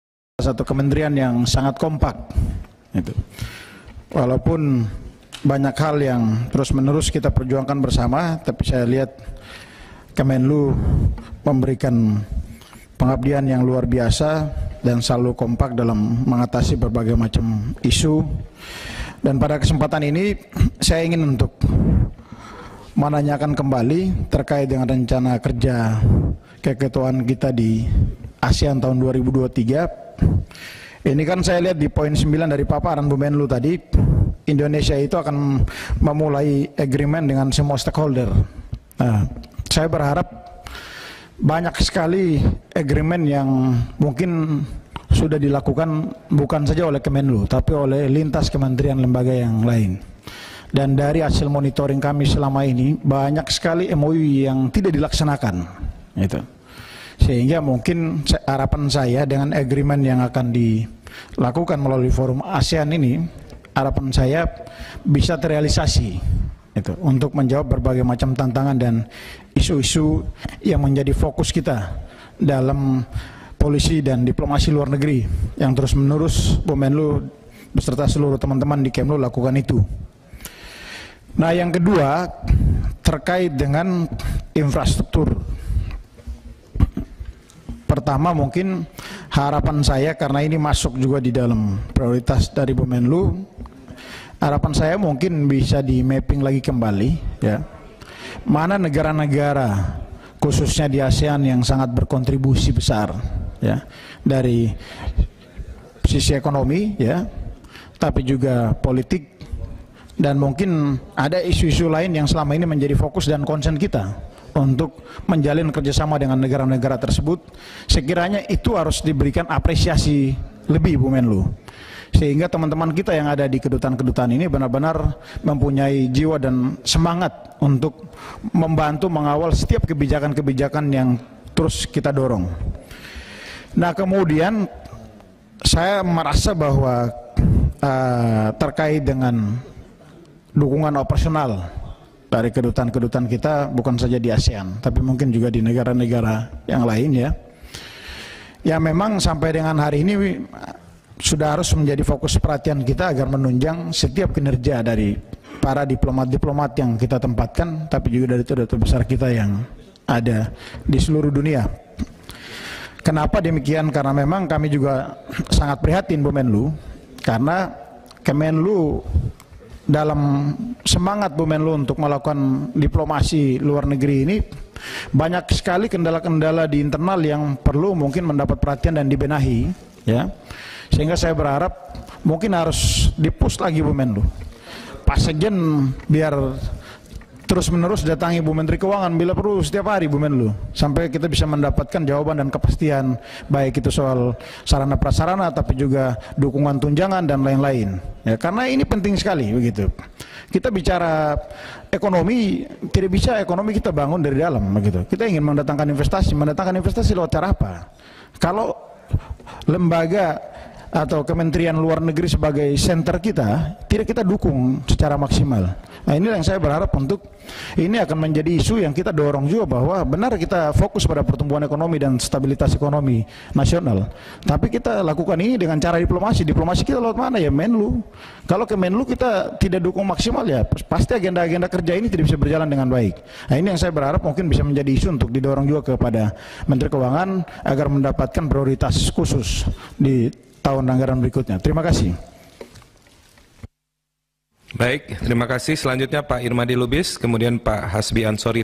YAN - TERKAIT RENCANA KERJA KEKETUAAN INDONESIA DI ASEAN 2023 INI
ANGGOTA KOMISI I DPR RI, YAN PERMENAS MANDENAS, S. Sos., M.Si - TERKAIT RENCANA KERJA KEKETUAAN INDONESIA DI ASEAN 2023 INI, GERINDRA: BERDASARKAN MONITORING KAMI BANYAK SEKALI AGREEMENT YANG TIDAK DILAKSANAKAN SEBELUMNYA, KITA BERHARAP KEDEPAN BISA TEREALISASI DENGAN BAIK